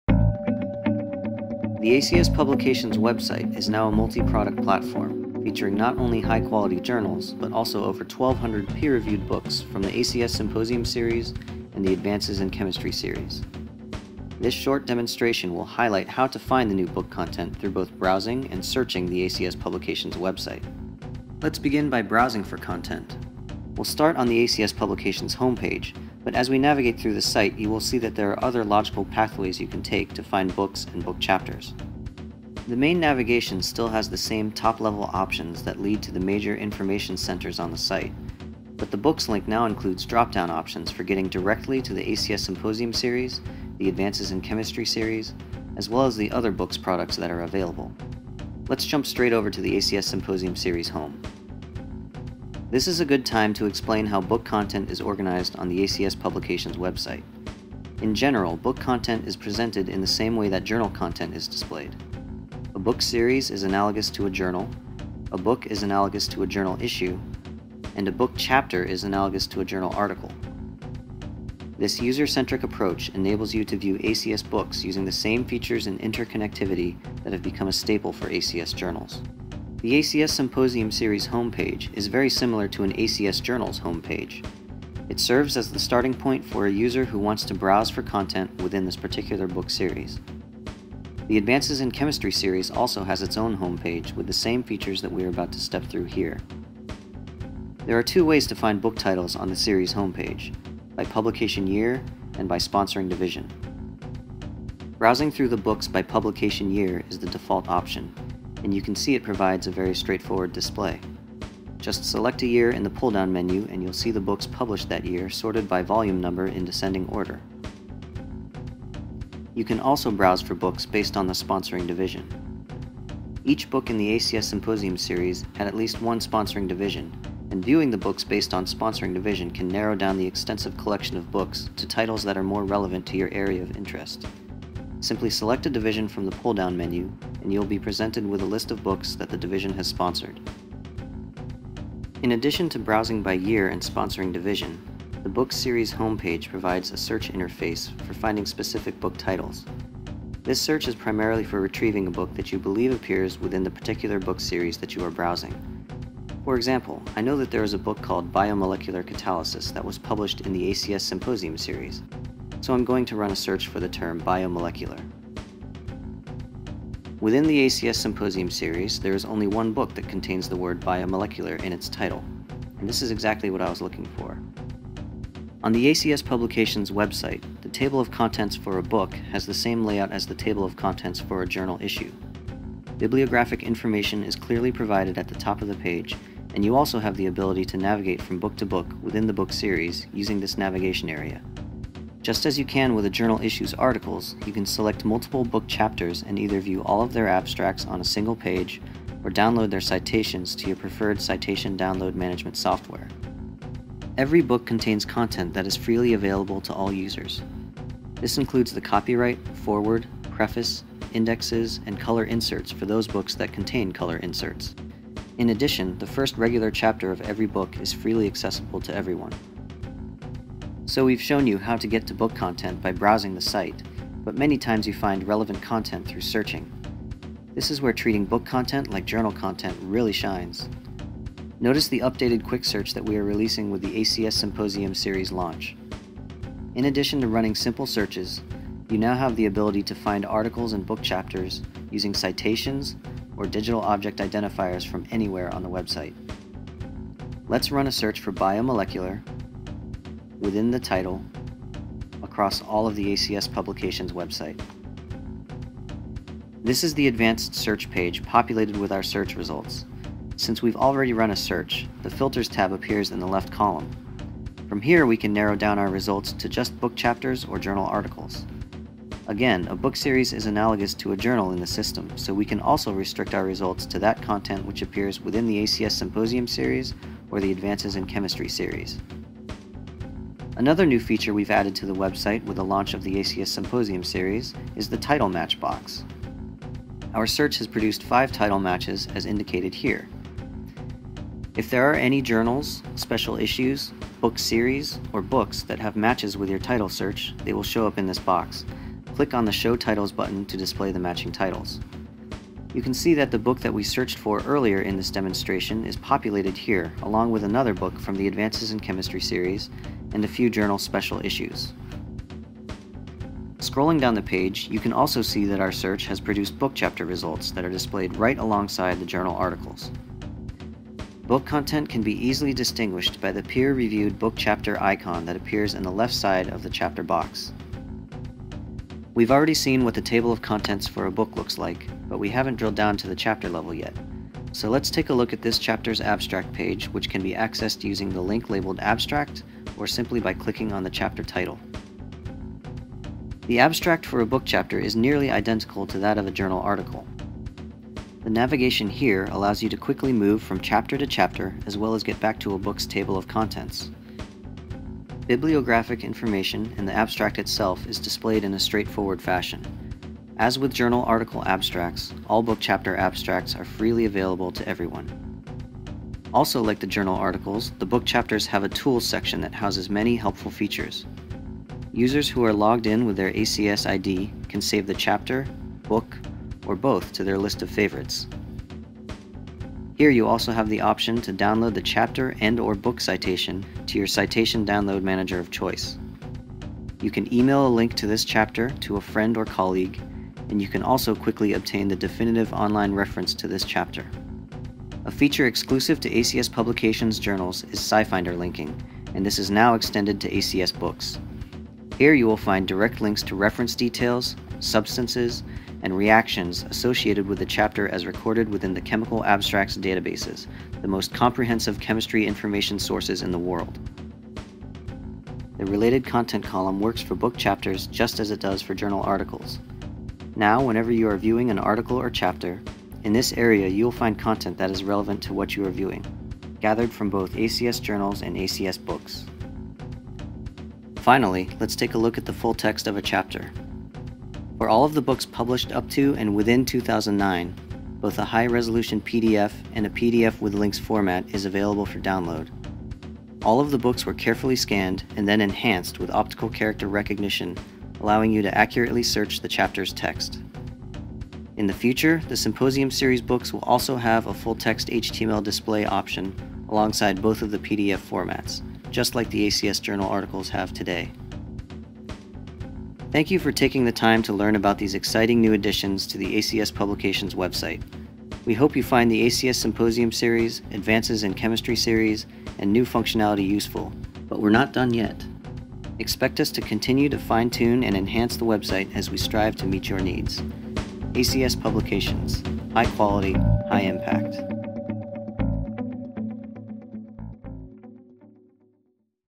ACS Books Demo
Demo of ACS Books, available on the same award-winning website as the journals of the American Chemical Society.